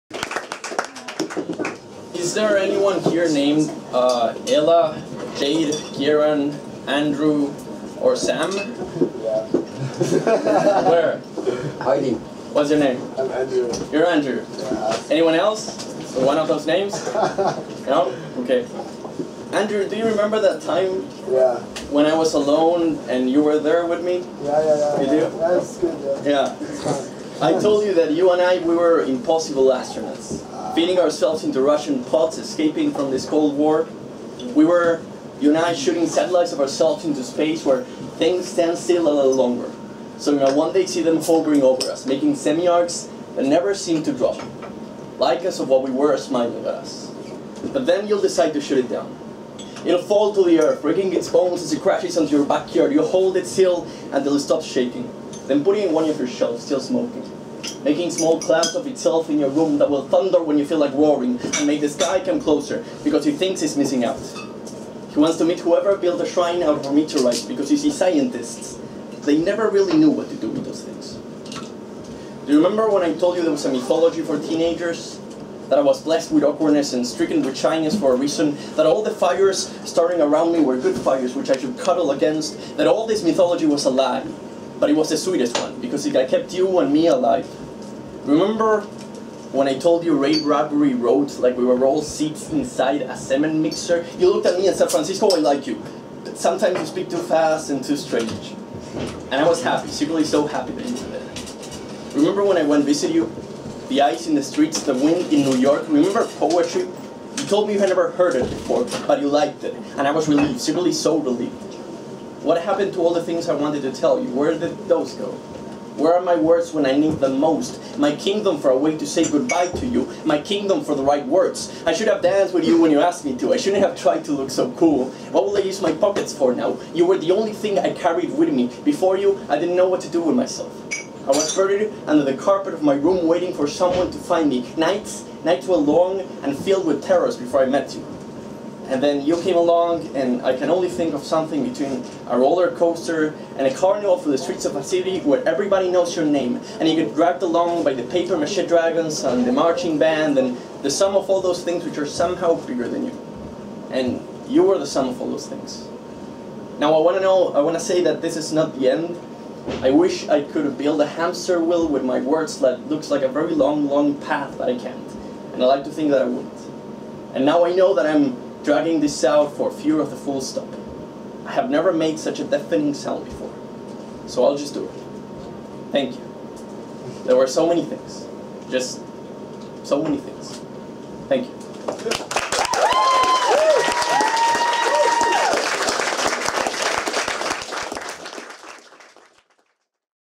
"Impossible astronauts", for Andrew
Performed in a slam venue in the basement of The International bar, Dublin.